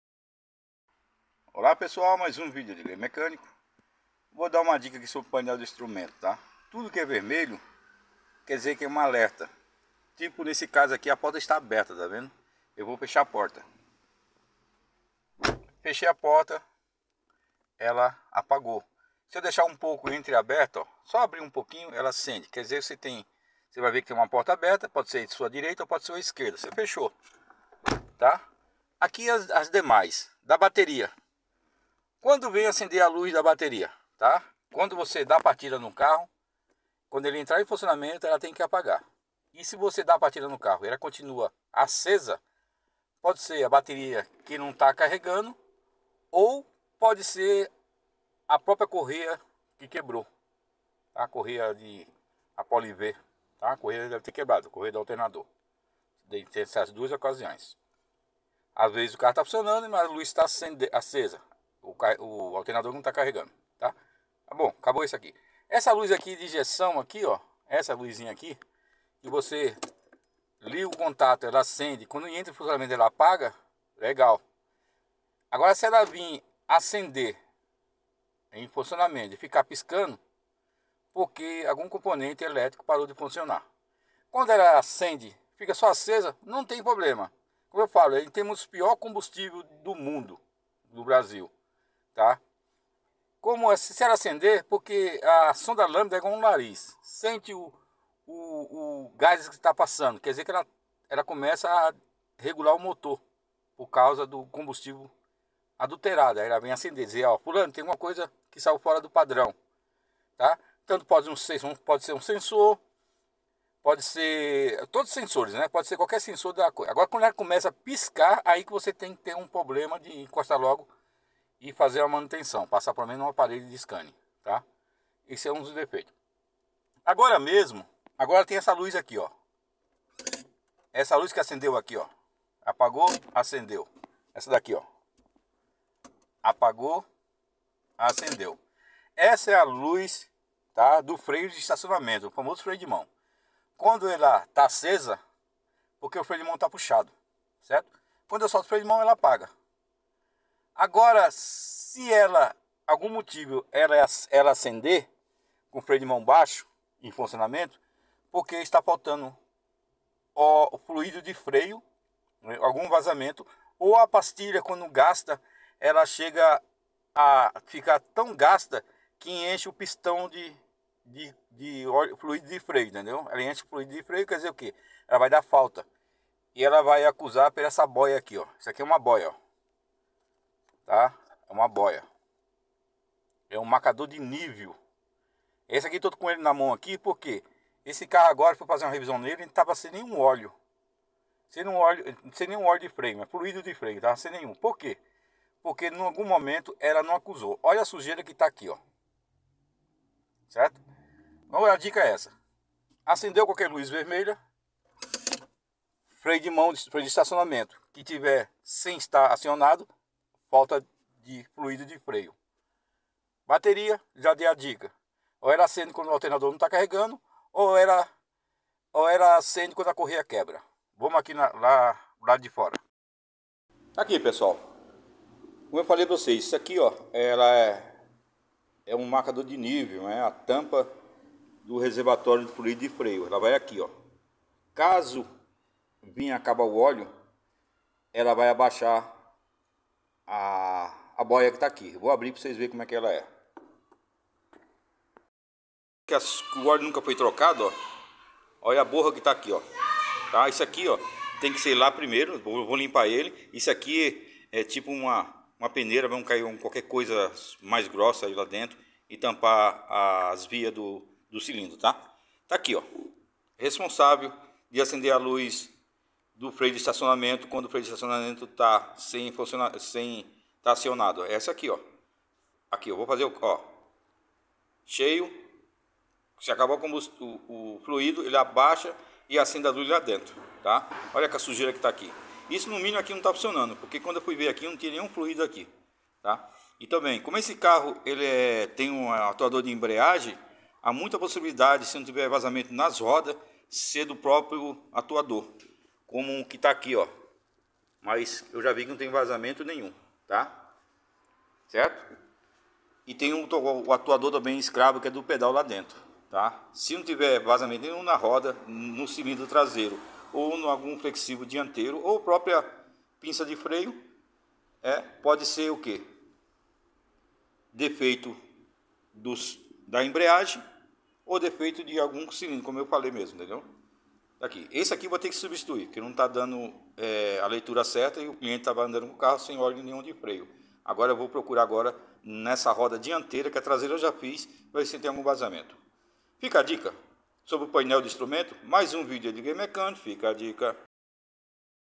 Painel de instrumento
Painel de instrumento,é muito importante traduzir os sinais de aviso...